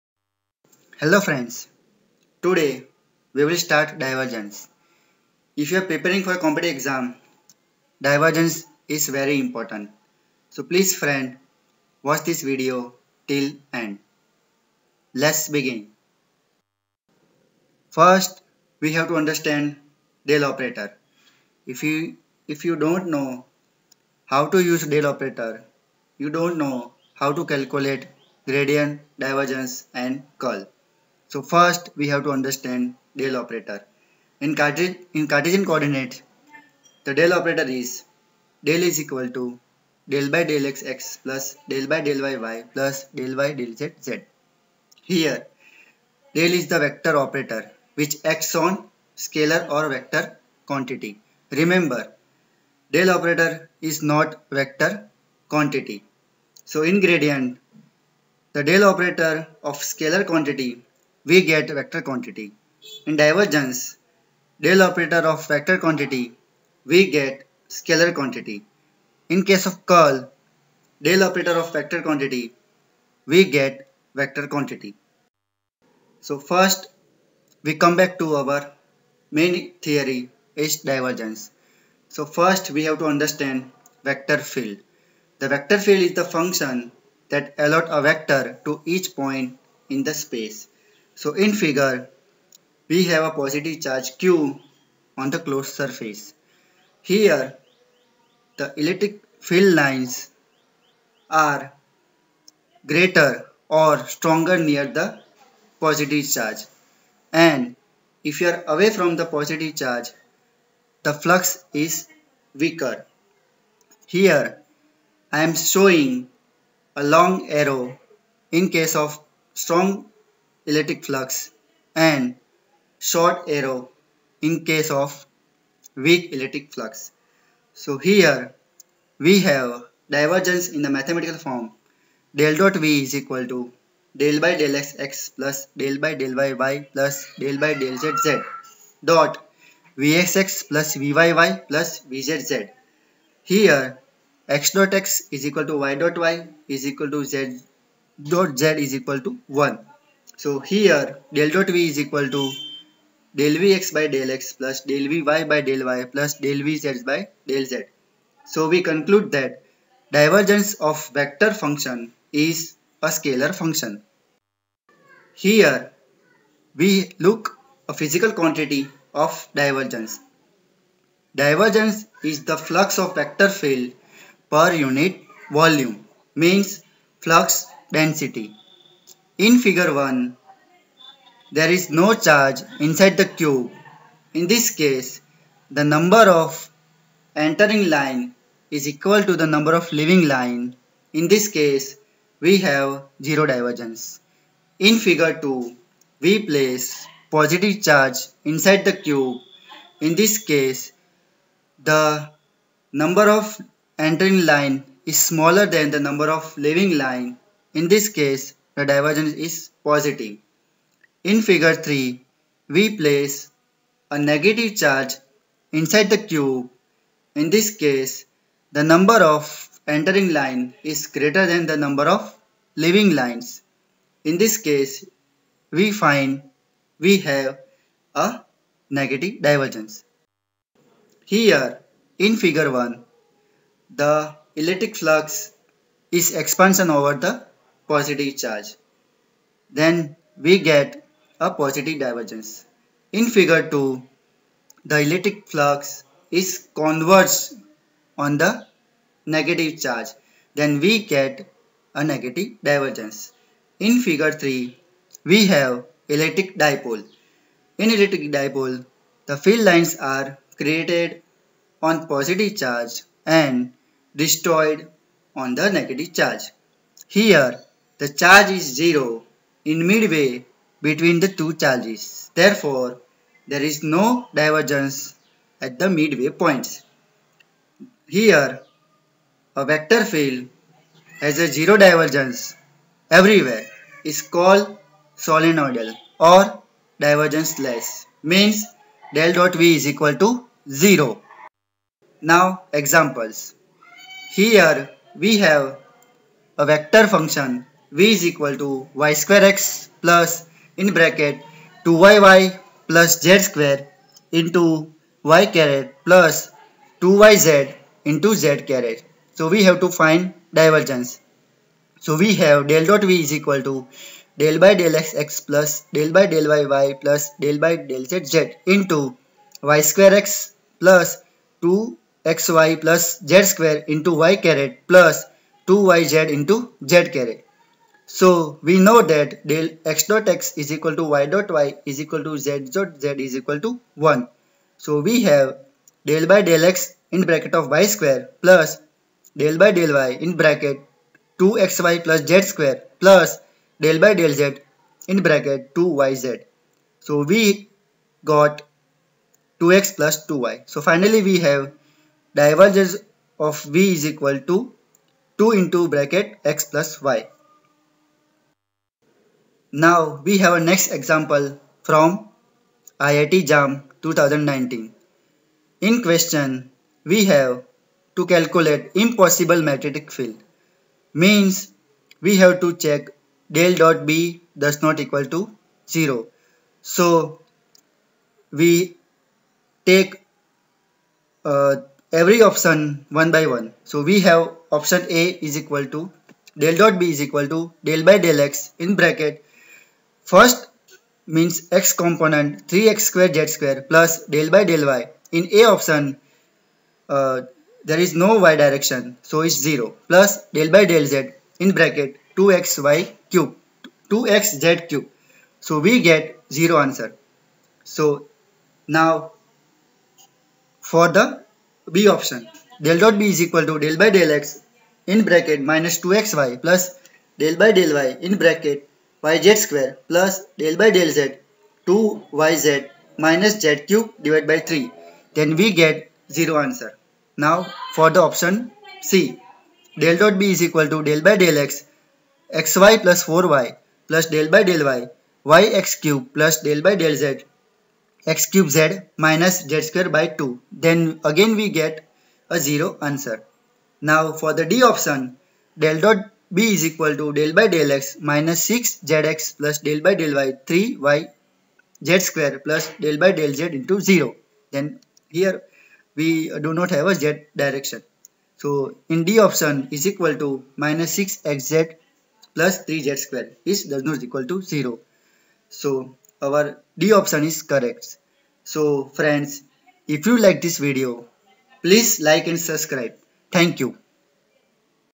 Divergence and its Problems (IIT-JAM)
concepts of divergence and problems from competitive exams.